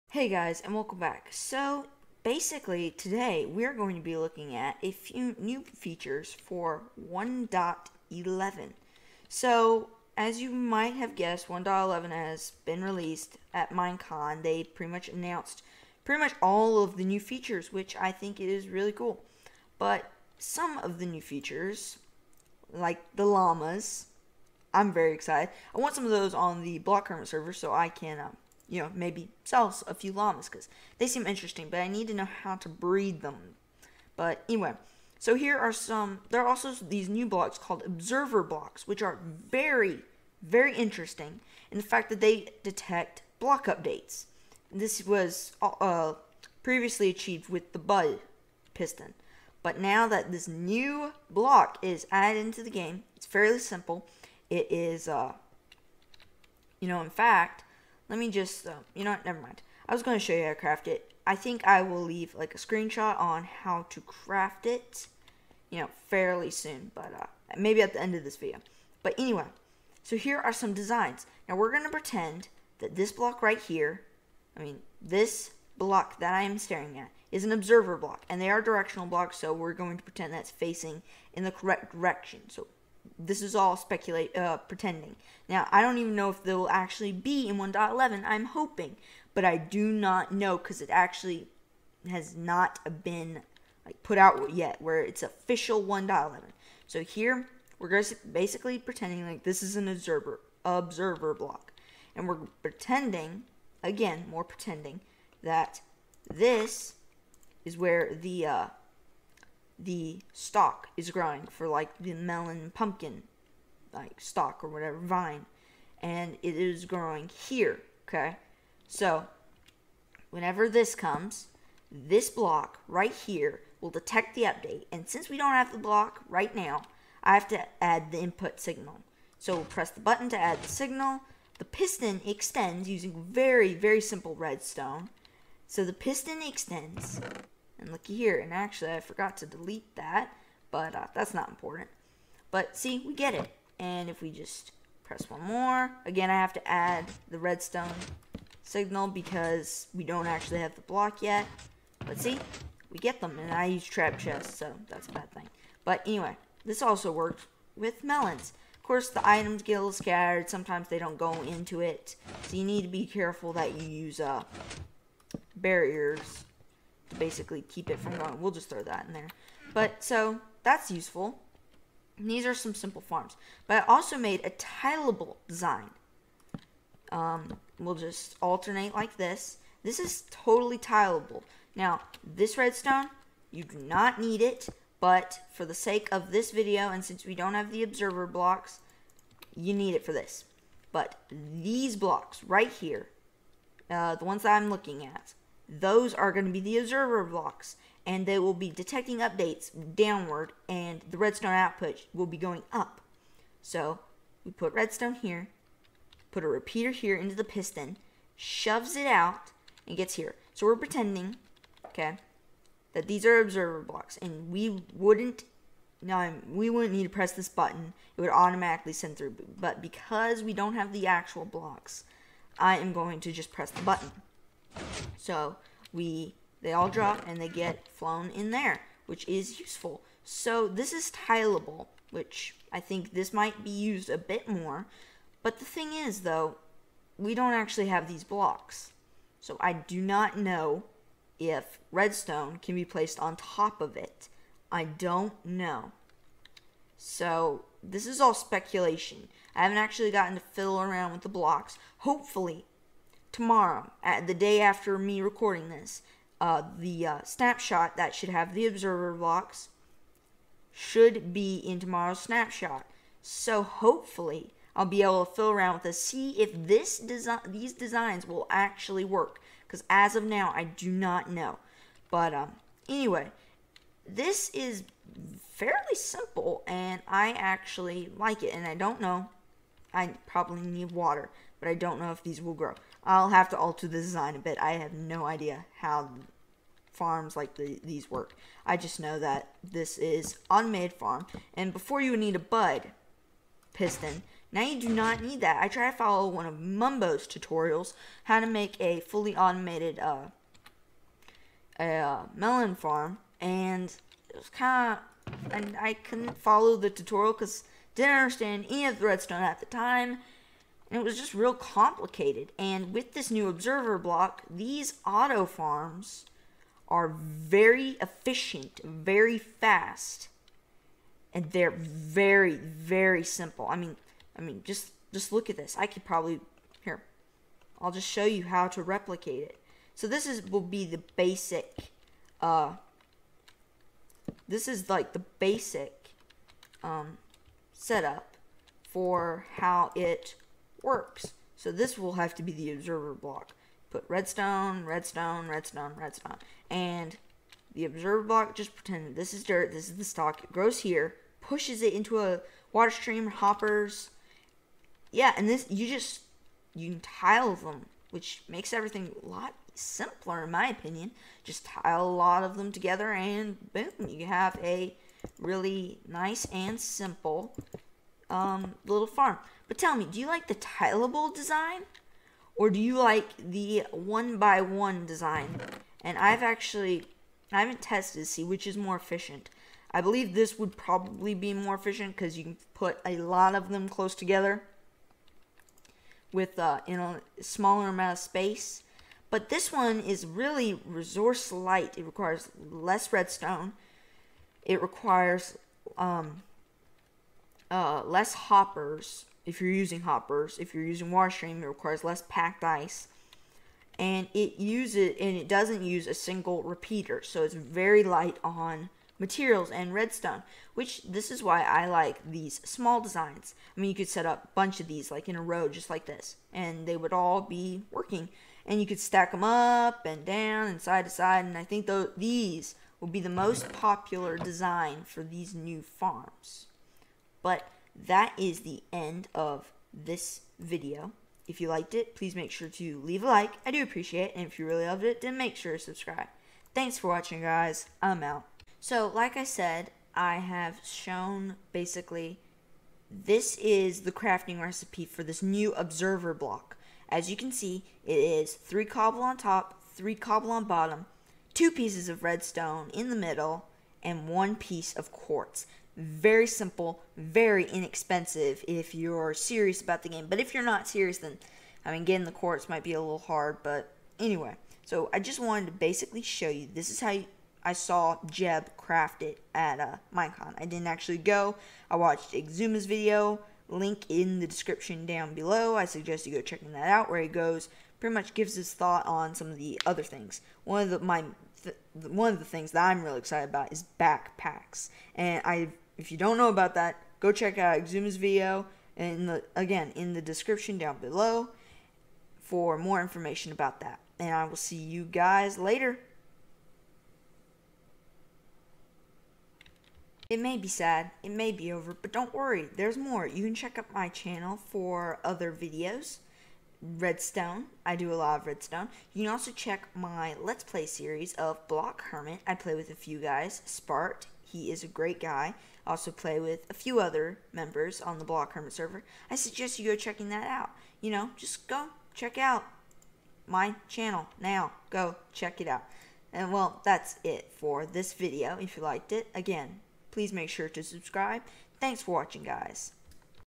Pre-1.11 Melon and Pumpkin Farm
In this Redstone video we will be looking at the announced observer block, and the potential farms for pumpkins, and melons.